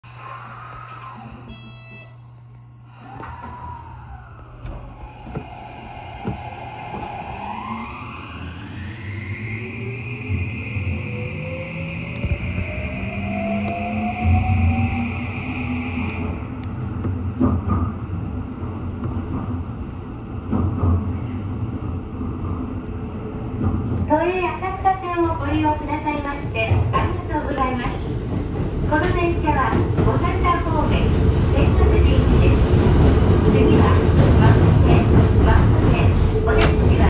【12-471F 馬込車両検修場出場回送（全検）】西馬込駅2番線に停車中の都営E5000形電気機関車E5001編成＋都営12-000形12-471編成を追い抜くシーン（2499T）2024.3.26
終電で先回り👍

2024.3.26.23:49